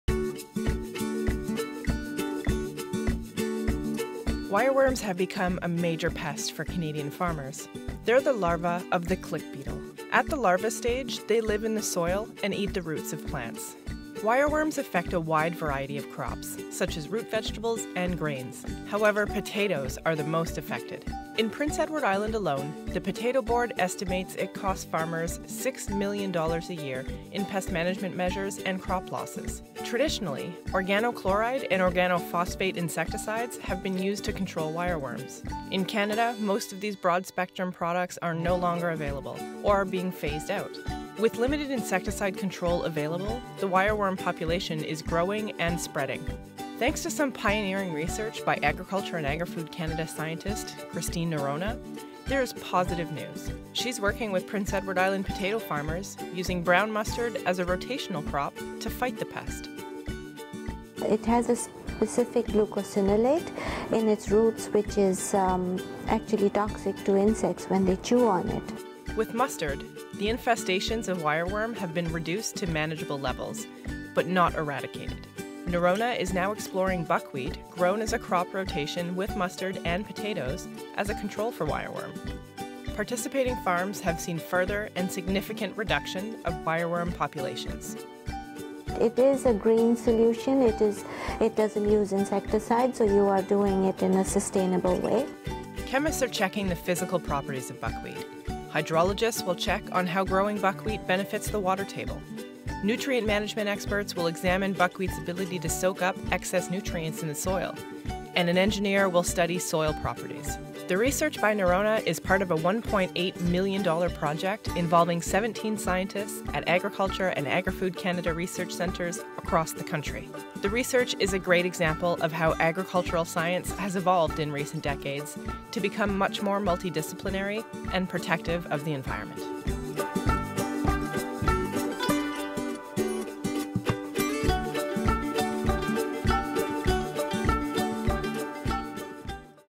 New Pest Management Practices for the Control of Wireworm
AAFC scientists from across the country have been working hard to provide farmers with a natural method to control wireworm infestations and reduce crop losses.

Full Video Transcript:

Narrator: Wireworms have become a major pest for Canadian farmers. They are the larvae of the click beetle. At the larvae stage, they live in the soil and eat the roots of plants. Wireworms affect a wide variety of crops, such as root vegetables and grains. However, potatoes are the crop most affected. In Prince Edward Island alone, the Potato Board estimates it costs farmers six million dollars a year in pest management measures and crop losses.  Traditionally, organochlorine  and organophosphate insecticides have been used to control wireworms. In Canada, many of these broad-spectrum products are no longer available or are being phased out. With limited insecticide control available, the wireworm population is growing and spreading. Thanks to some pioneering research by Agriculture and Agri-Food Canada’s scientist, Dr. Christine Noronha, there is positive news. She is working with Prince Edward Island potato farmers, using brown mustard as a rotational crop to fight the pest.
Christine Noronha: Brown mustard has very high levels of glucosinolate in its tissues and also in its roots. It has a specific glucosinolate in its root which is actually toxic to insects when they chew on it.
Narrator: With mustard, the infestations of wireworm have been reduced to manageable levels, but not eradicated. It may be that other rotational crops need to be added to the mix. Noronha is now exploring buckwheat grown in a crop rotation with mustard and potatoes as a control for the wireworms. She is seeing some good results. Participating farms have seen further and significant reductions of wireworm populations.
Christine Noronha: It is a green solution. It reduces the   use of insecticides and you are doing it in a sustainable way.
Narrator: Chemists are checking physical properties of buckwheat. Hydrologists will check on how growing buckwheat benefits the water table. Nutrient management experts will examine buckwheat’s ability to soak up excess nutrients in the soil. And an engineer will study soil properties.
Christine Noronha: We are looking at all aspects of buckwheat. We are looking at the production, production for seed, how it controls wireworm, the different species of wireworms. We are looking at it as a whole picture of buckwheat in relation to wireworm control.

Narrator: The research by Noronha is part of a $1.8 million dollar project involving 17 scientists at Agriculture and Agri-Food Canada research centres across the country.
The research is a great example of how agricultural science has evolved in recent decades to become much more multidisciplinary and protective of the environment. Agricultural scientists don’t work in silos -- they just want to help farmers produce a good crop with minimum impact to the environment.